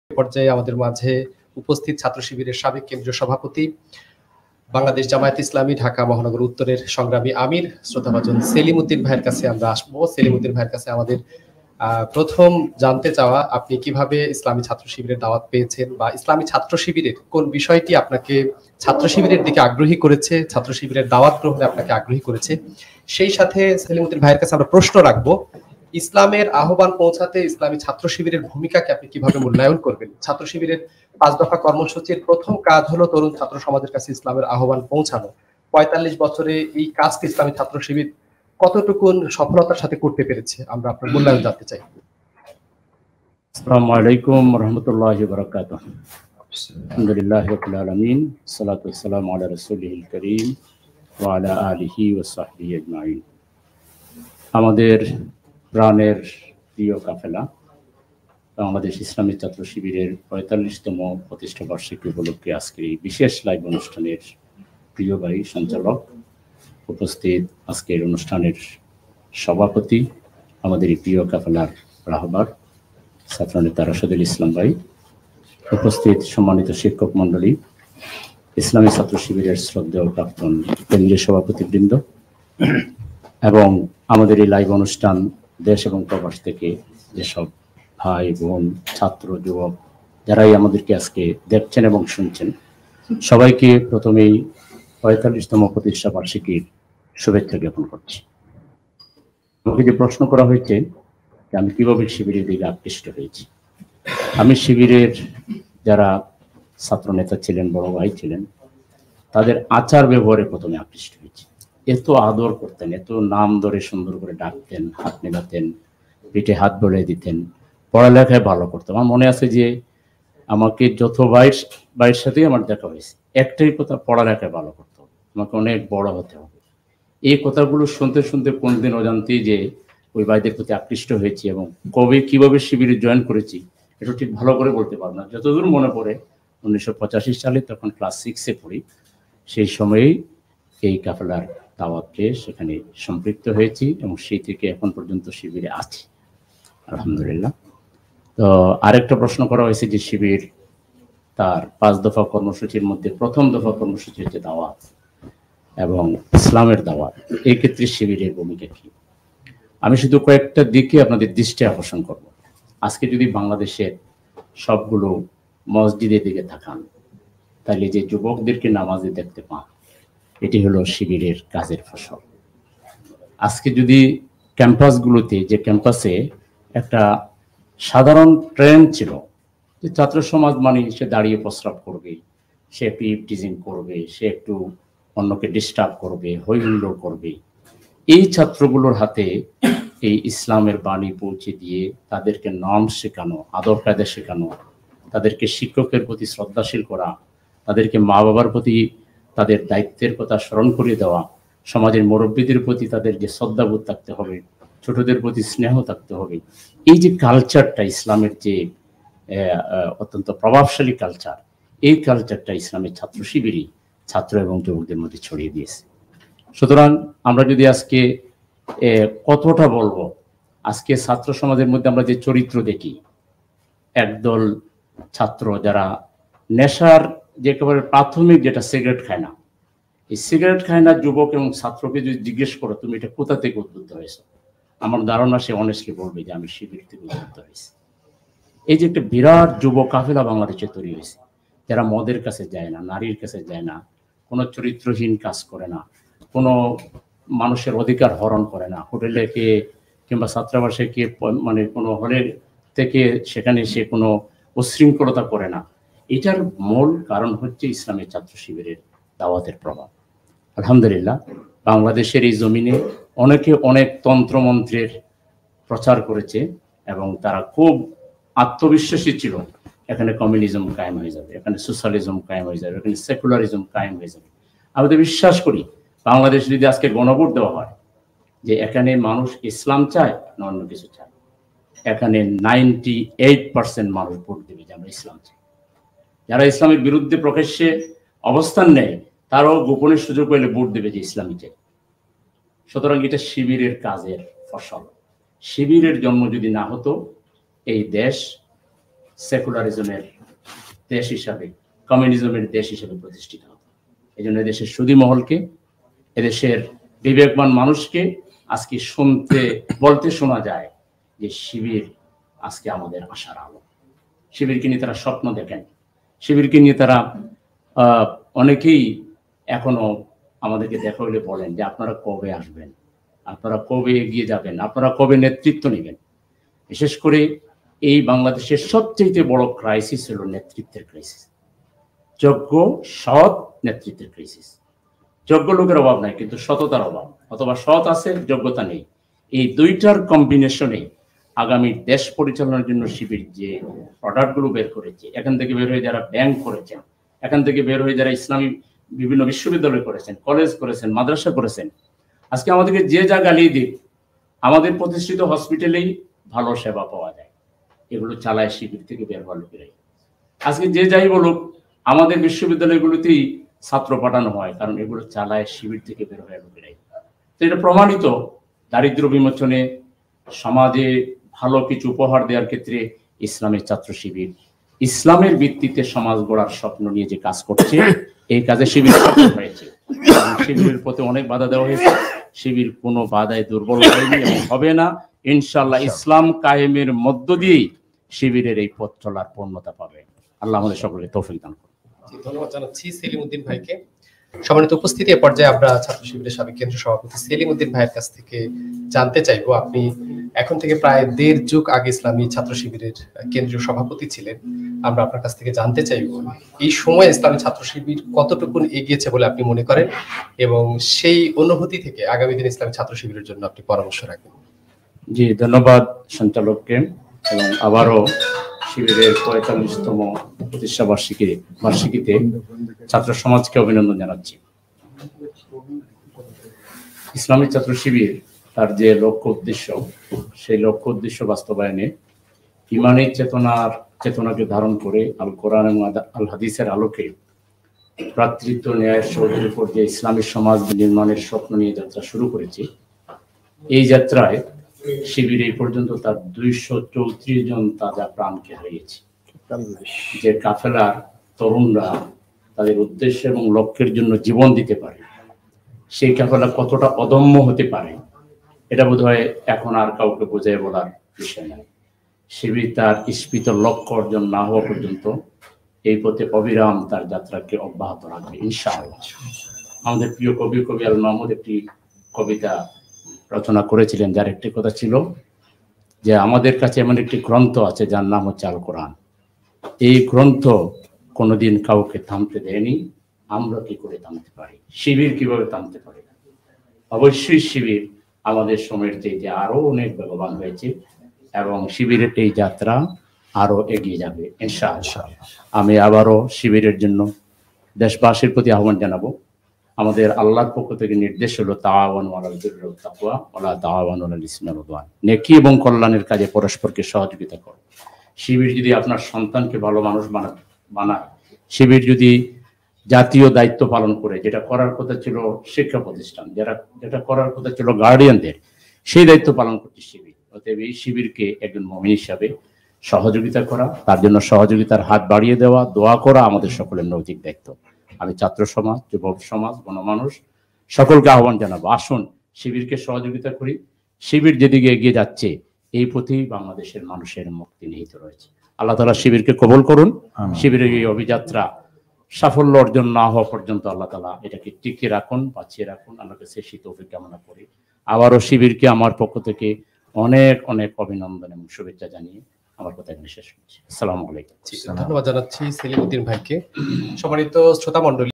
ছাত্রশিবিরের পথচলার ৪৫তম বর্ষ উপলক্ষ্যে বিশেষ লাইভ অনুষ্ঠান।। মুহাম্মদ সেলিম উদ্দিন
বাংলাদেশ ইসলামী ছাত্রশিবিরের পথচলার ৪৫তম বর্ষ উপলক্ষ্যে বিশেষ লাইভ অনুষ্ঠানে বক্তব্য রাখেন ছাত্রশিবিরের সাবেক কেন্দ্রীয় সভাপতি মুহাম্মদ সেলিম উদ্দিন।